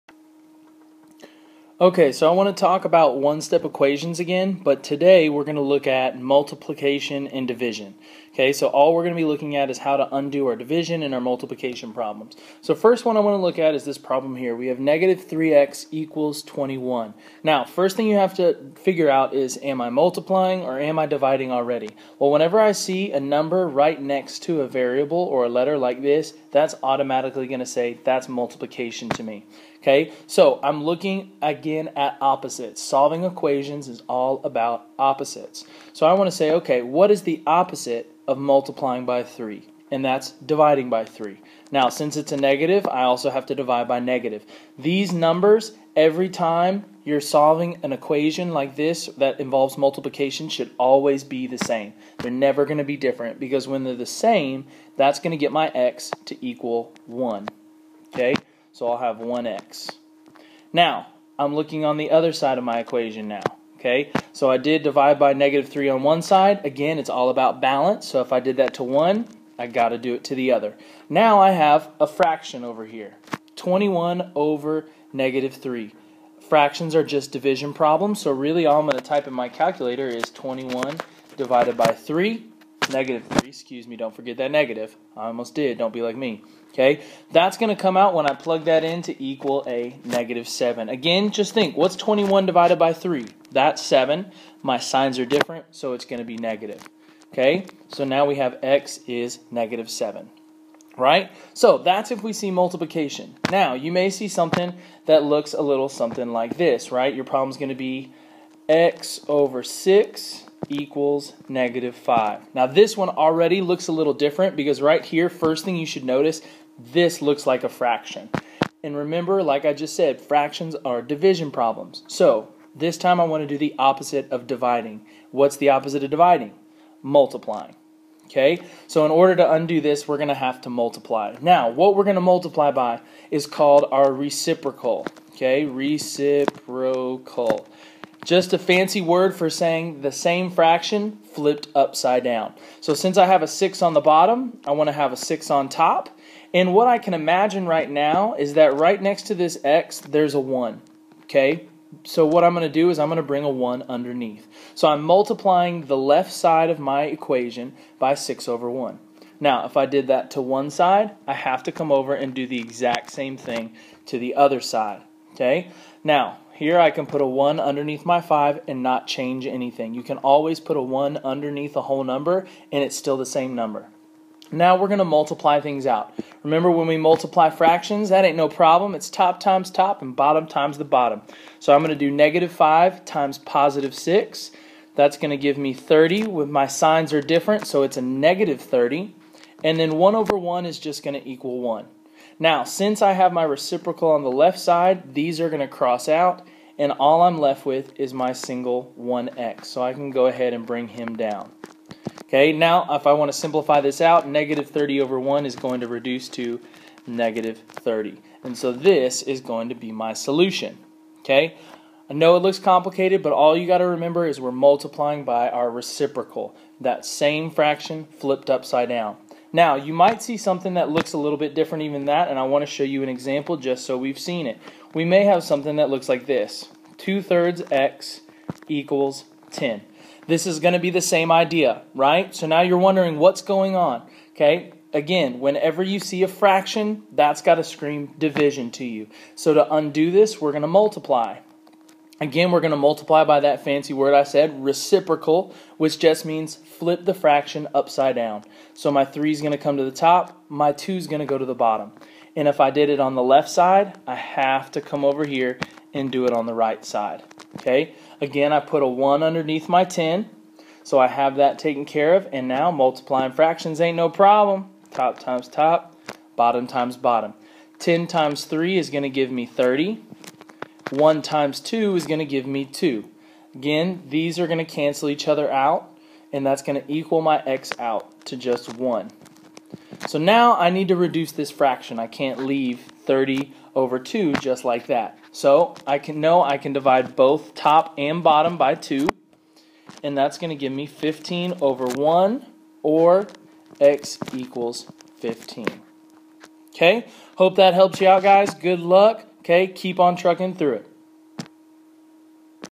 One Step Equations Multiplication and Division
A short video breaking down how to solve one step equations with multiplication and division